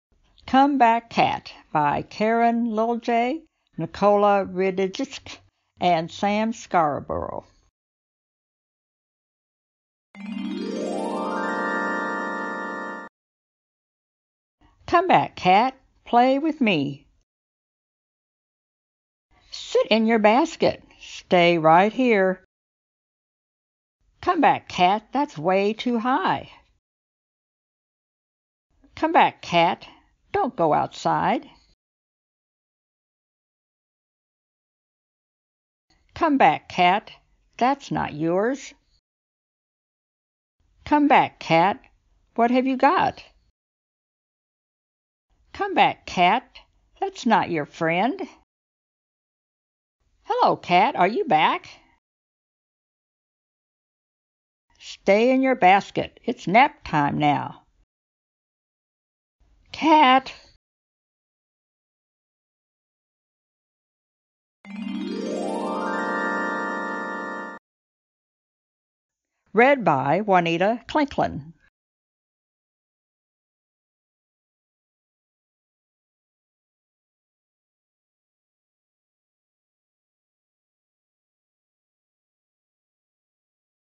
[1.0] - Come Back, Cat! - (video story book @ Anglihel.com)
"Come Back, Cat!"
by:
Illustrated by Karen Lilje
Written by Nicola Rijsdijk
Designed by Sam Scarborough

A delightful tale about an independent cat.
No cats were harmed in the production of this book.

Ready by Juanita Klenklen

"Come back, cat!" by Karen Lilje and Nicola Rijsdijk

and is available on this YouTube channel or at Anglihel.com